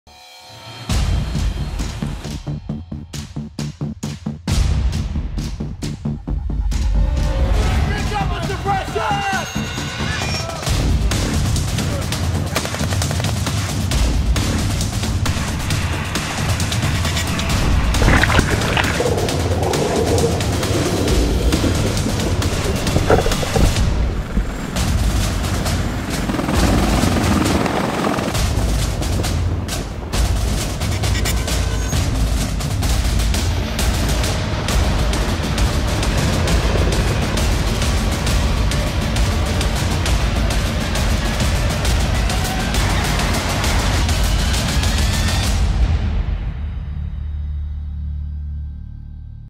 Marines Train for Upcoming Deployment
Warning: Extremely Motivating Content

Marines with the 22nd Marine Expeditionary Unit go through the seven-month gauntlet of pre-deployment training. The Marines practiced everything from boarding vessels to simulated helo crashes.

Video by: Cpl. Joshua Rudy